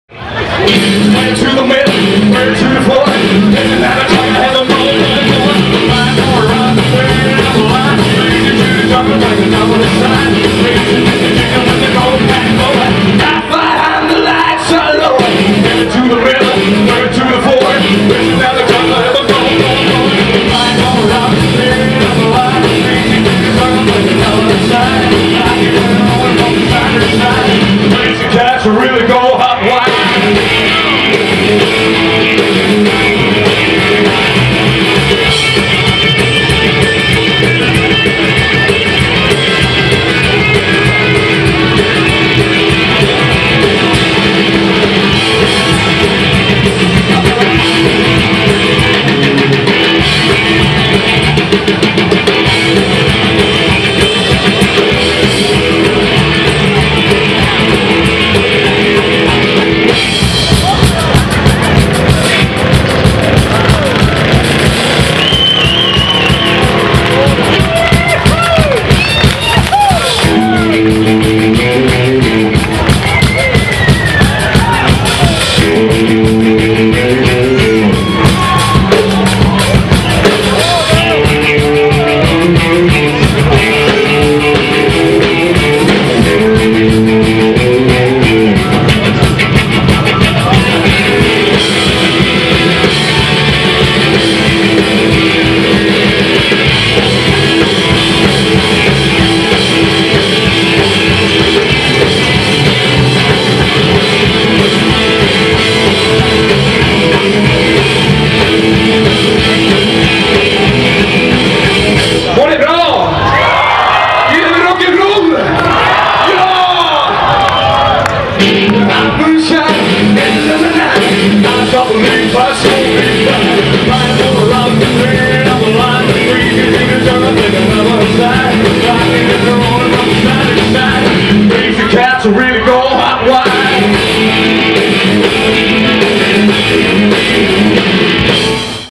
John Lindberg Rockabilly Trio (Sandviken 2005)
Inspelad på Jerntorget i Sandviken i samband med att Ghost Town Cruisers anordnade en cruising i stan. If you know the title of the song, please post it.